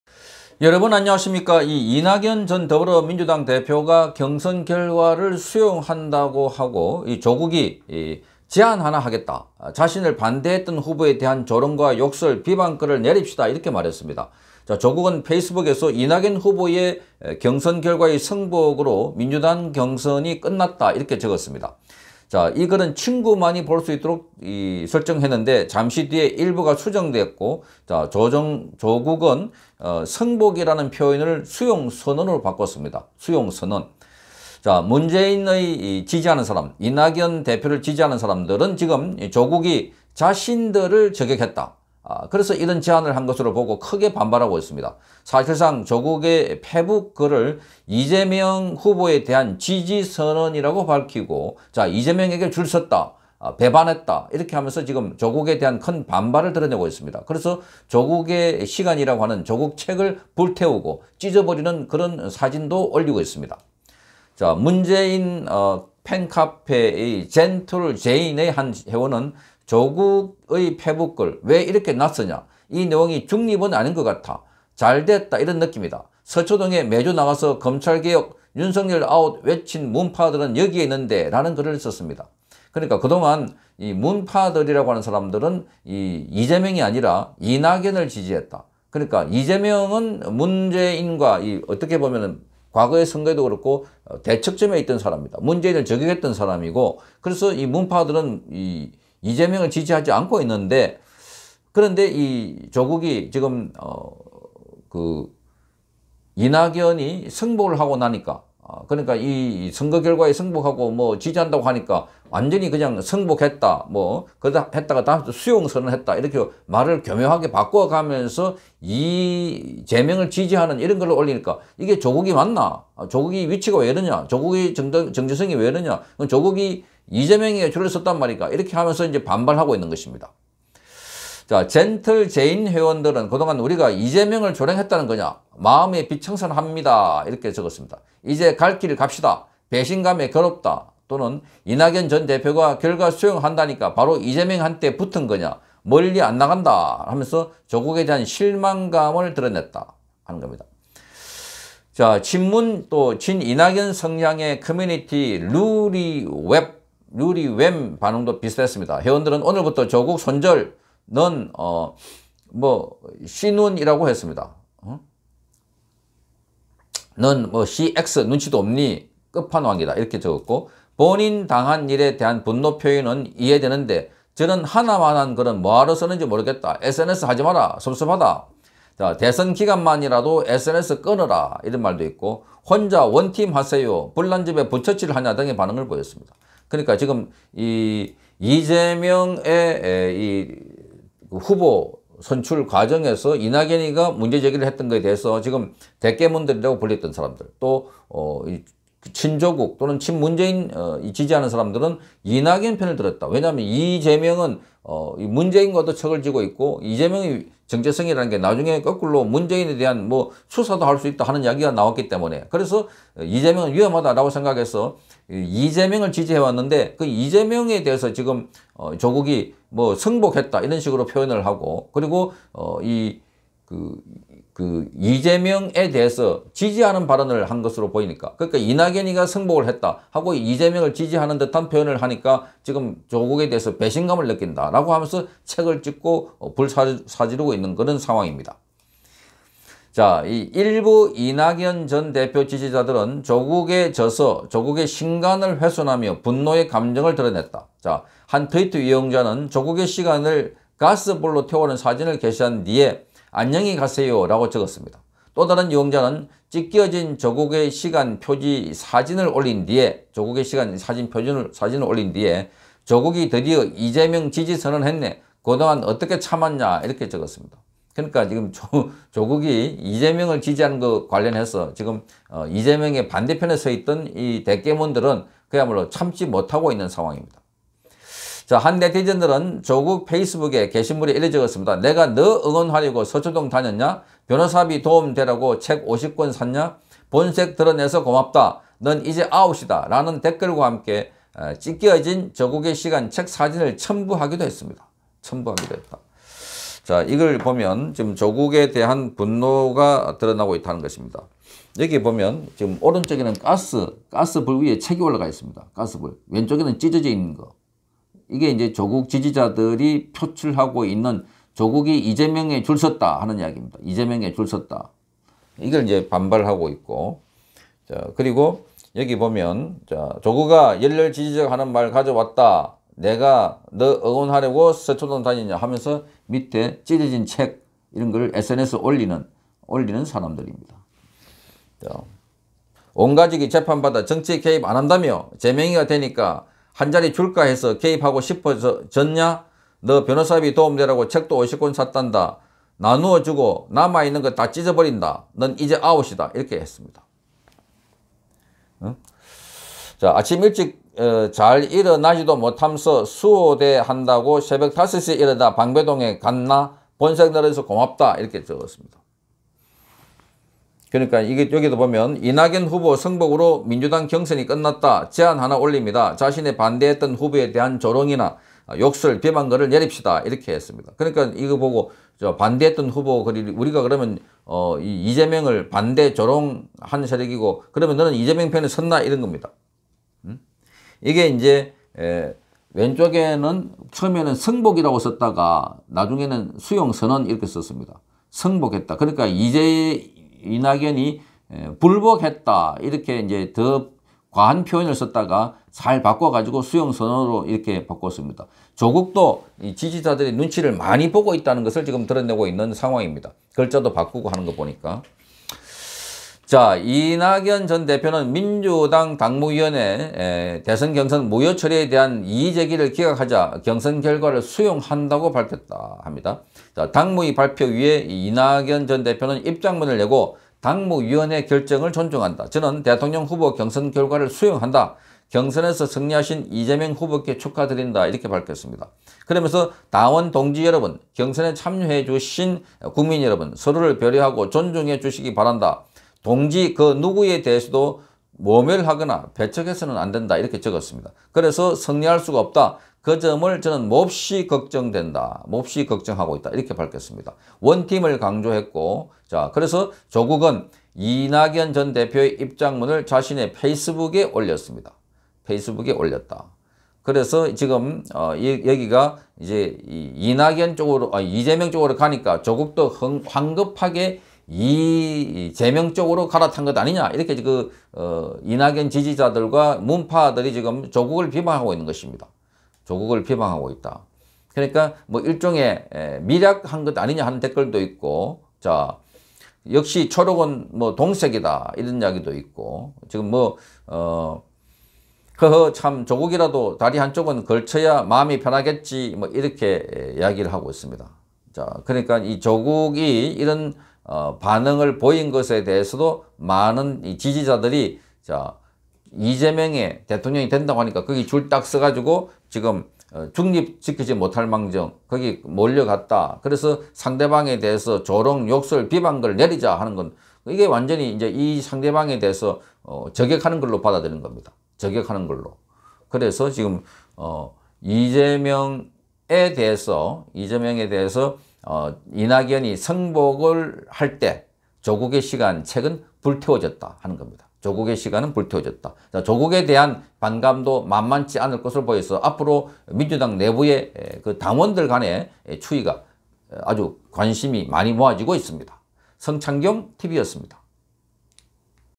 조국 “이낙연, 경선결과 승복” 글에…'조국의 시간’ 불태워졌다. 이재명에 줄섰다가 맹폭 당한 조국
조국 “이낙연, 경선결과 승복” 글에…'조국의 시간’ 불태워졌다